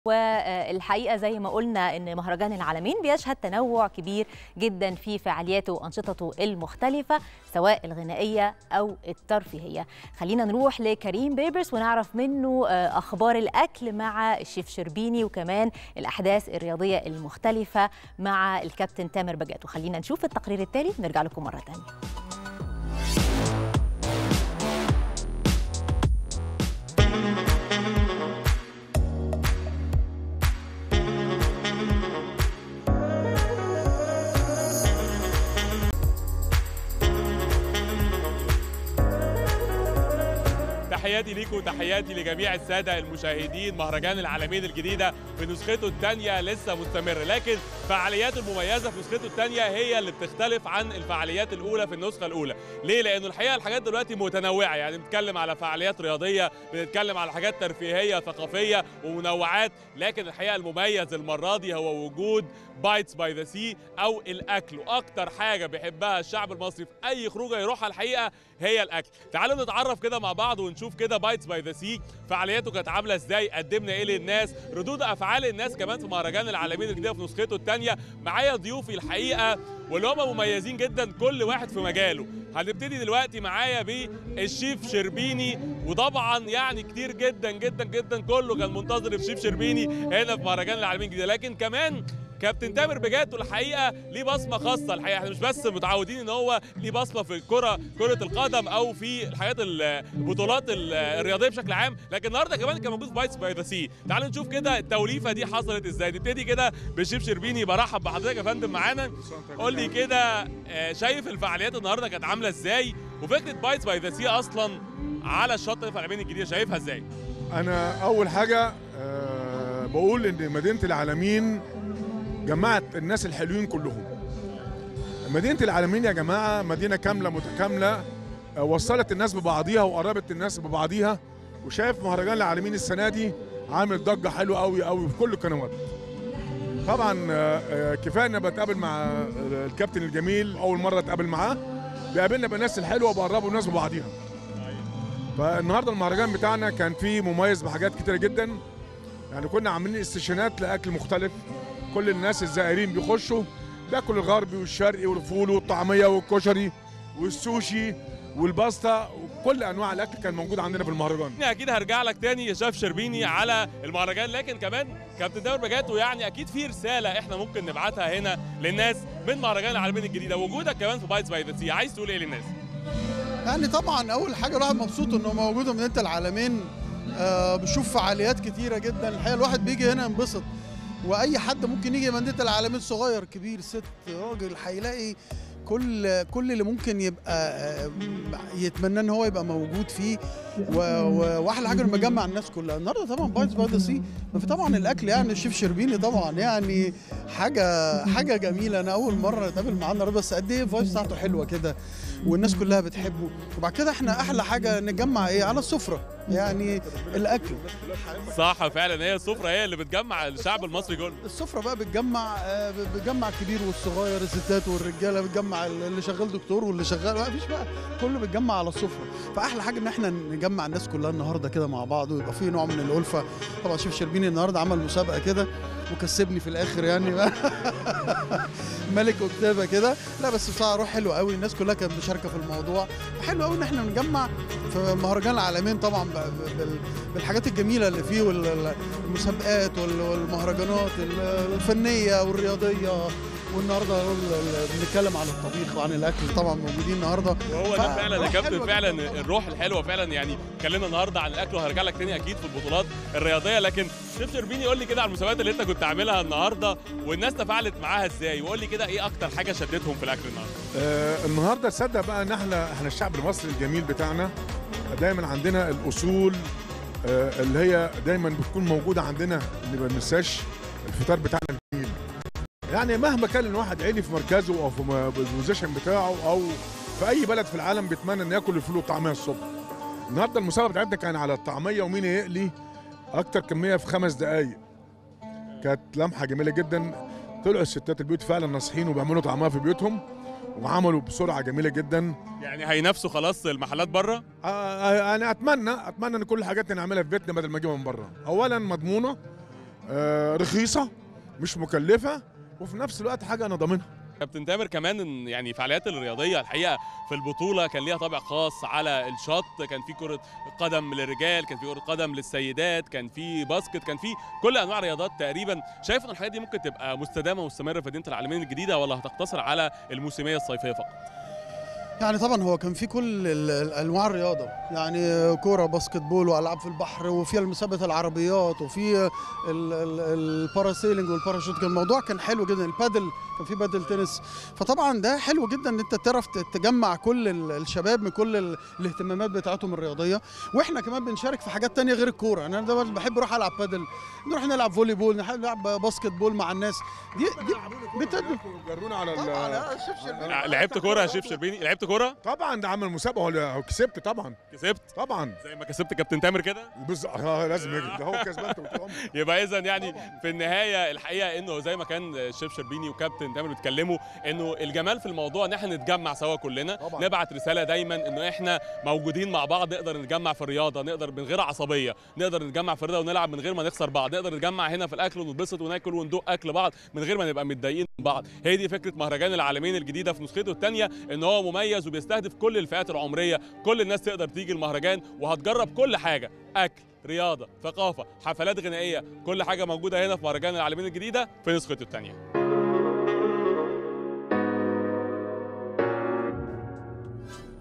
حوار خاص لـ كاميرا 📽"عالم العلمين" 🌍مع الشيف 👨‍🍳الشربيني وتامر بجاتو من مهرجان العلمين الجديدة⛱
حوار خاص لـ كاميرا 📽"عالم العلمين" 🌍مع الشيف 👨‍🍳الشربيني والكابتن تامر بجاتو من مهرجان العلمين الجديدة⛱مهرجان_العلمين_الجديدة